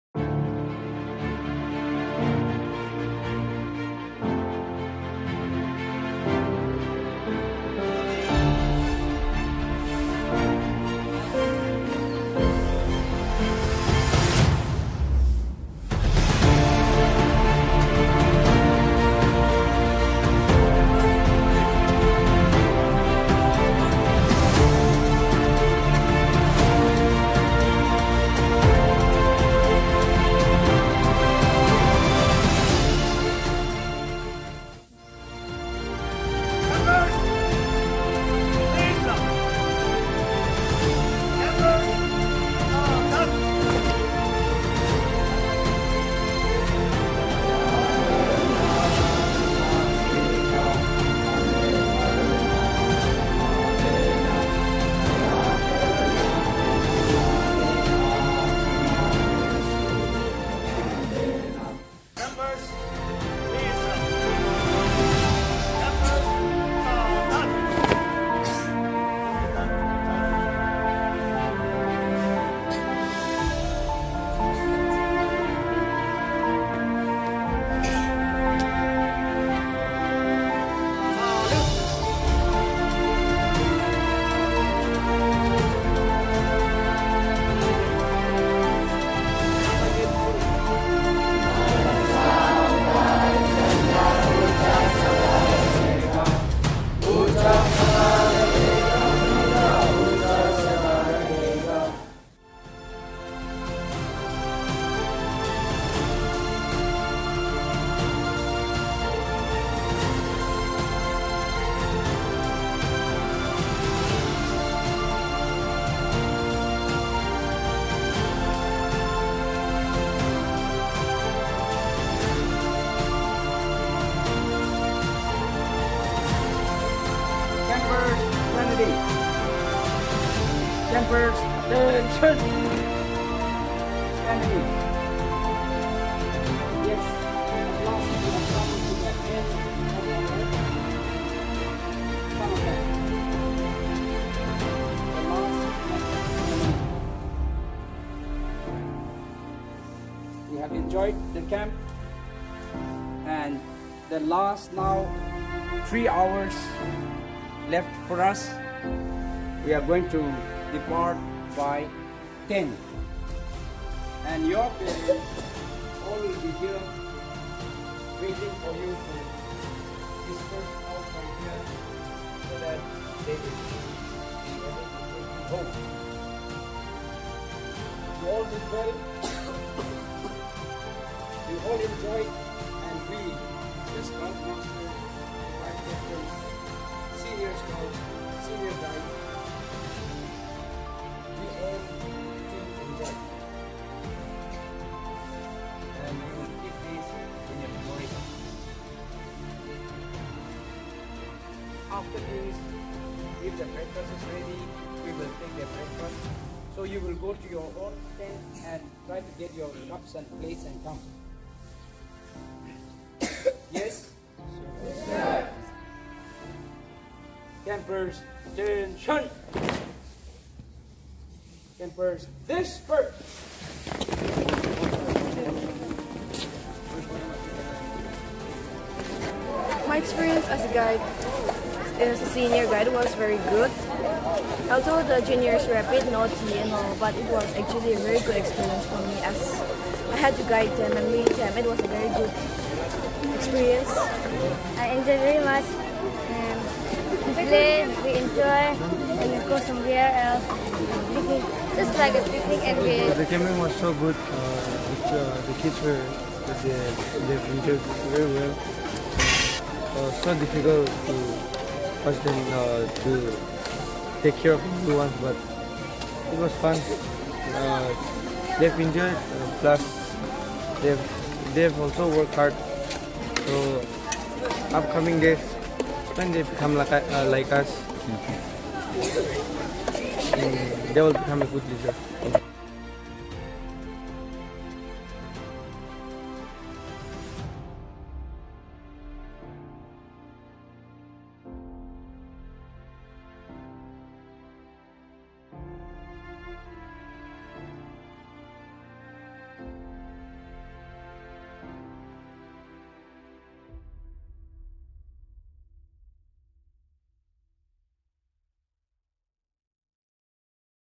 SUMMER CAMP | SCOUTS AND GUIDES | DON BOSCO HIGHER SECONDARY SCHOOL
Don Bosco Higher Secondary School and Holy Cross Higher Secondary School, Dimapur have jointly organised a summer camp for Scouts and Guides.